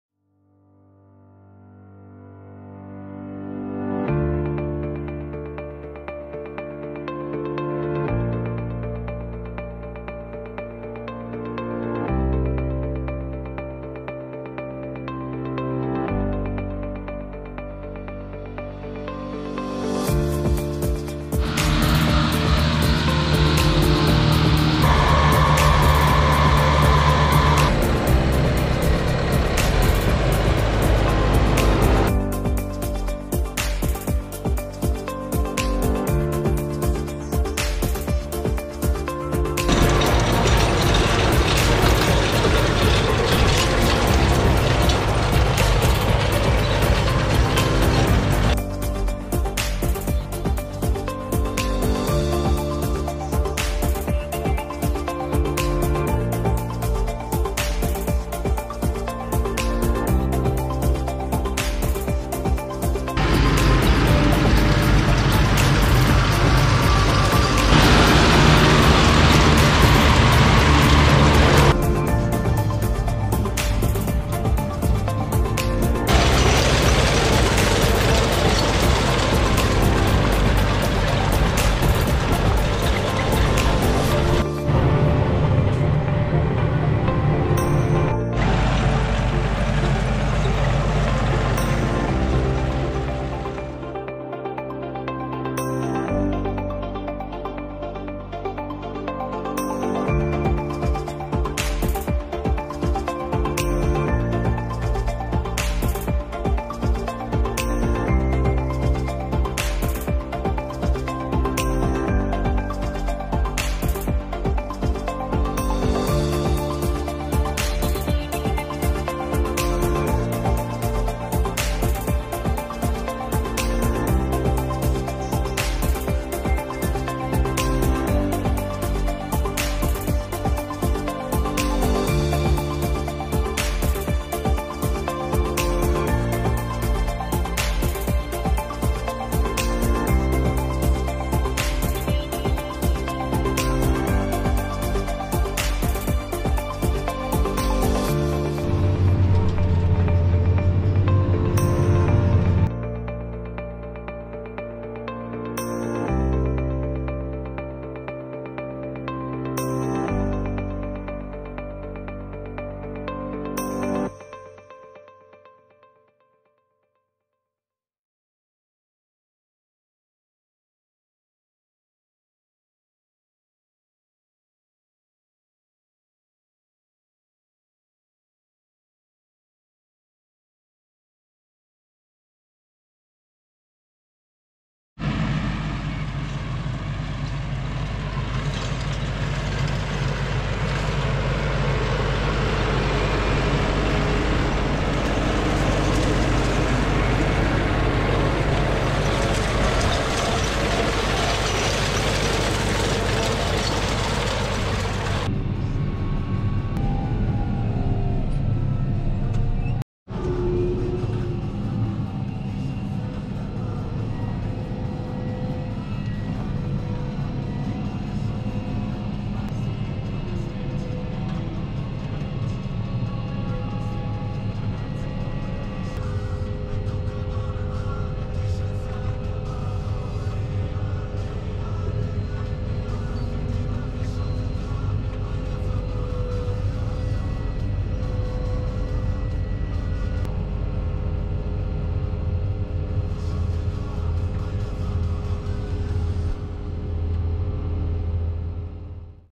1. Schnitt |2023|Fendt|Claas| und Krone etc. bei Nassen Wetterverhältnissen!
Es geht wieder los! Der erste schnitt steht bevor, wir trotzen auch bei sehr nassen Wetterverhältnissen!
Auf dem Silo wurde mit einem Claas Arion 460 festgefahren, und 2x Fendt Vario 828 und 724 mit Krone und Pöttinger Ladewägen wurde geladen.